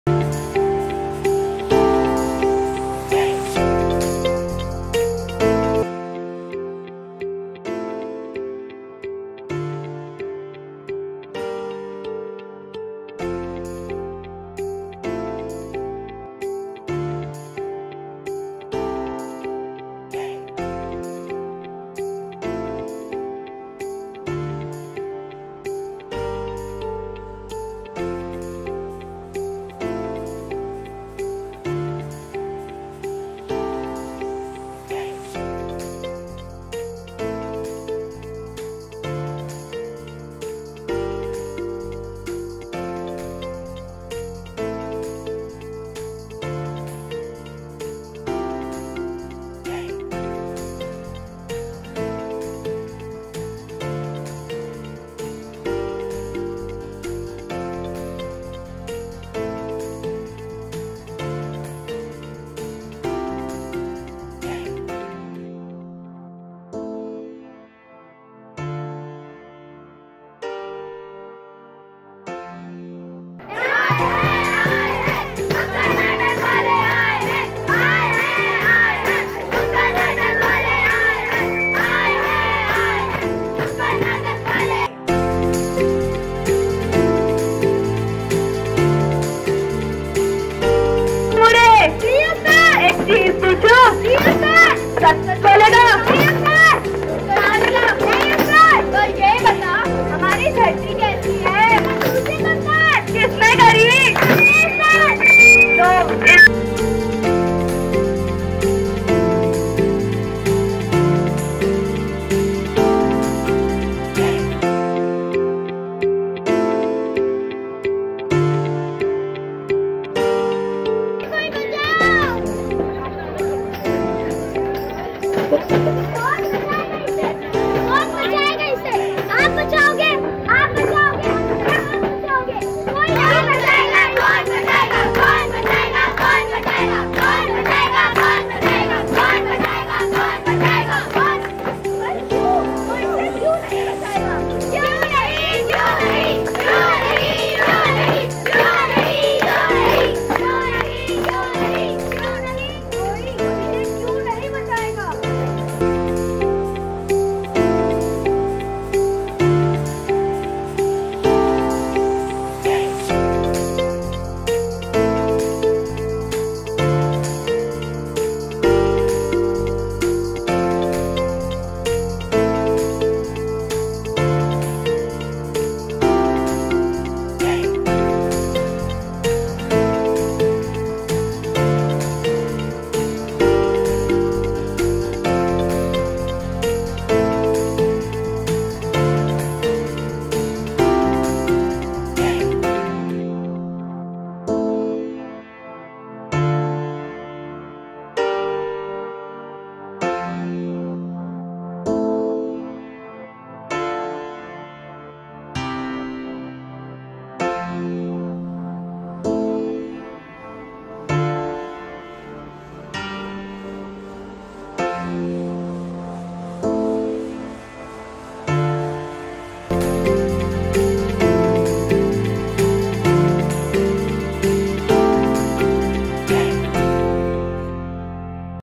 Marching for Mother Earth
Marching for Mother Earth: Advocating for a healthier planet

Together for a greener tomorrow: Students of Jesus Grace Modern School (Sr Sec) leading the charge on Earth Day